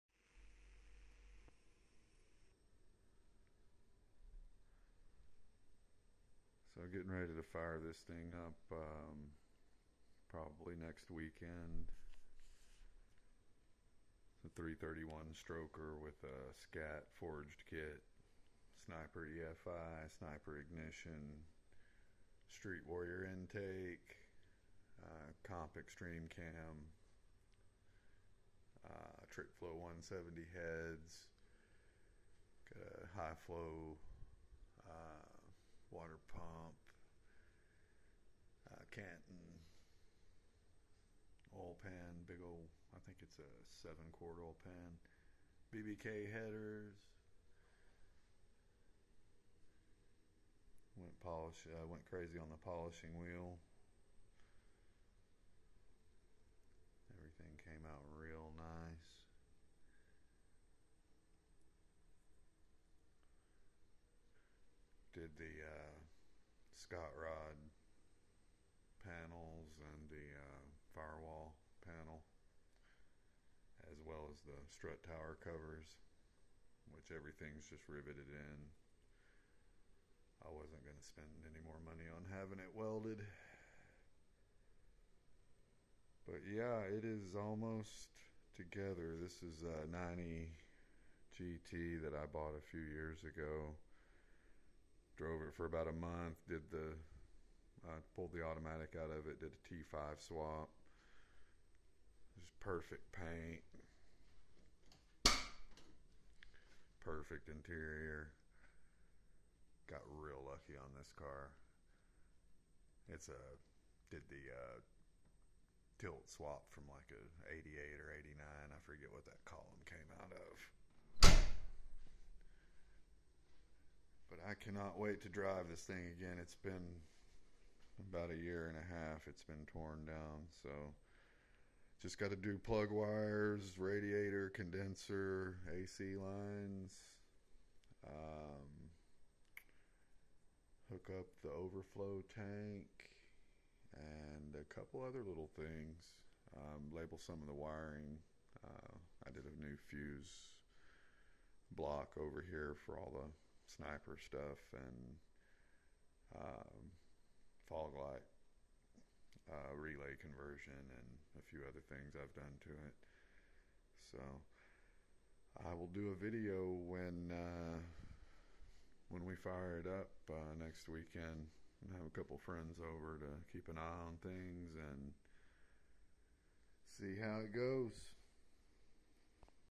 The Foxbody Mustang is almost done! Update on the 331 build.
Very little left to do.